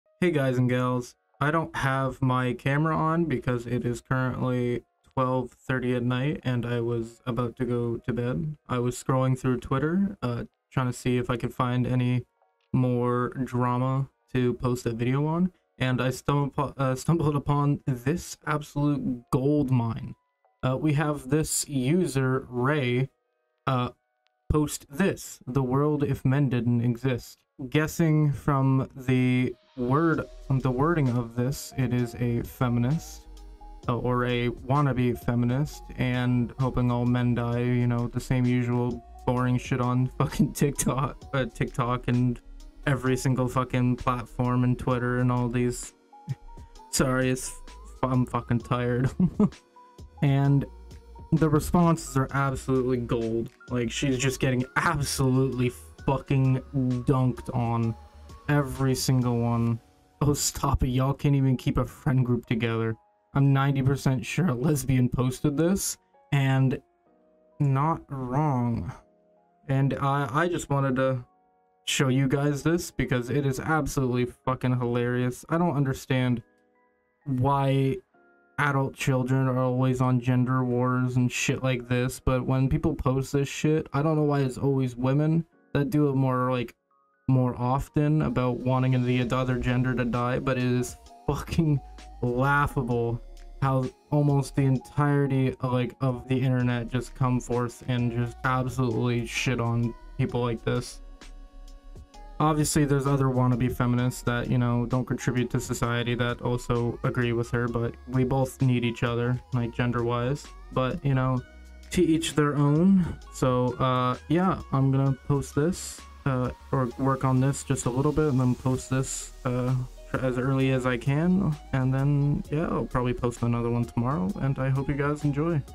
Feminist Wants All Men To Die
yuh, this is an actual person in real life. Why cant we all get along and act like adults and not bring gender into everything? i dont think its that hard tbh

If you are reading this why dont you follow please? :3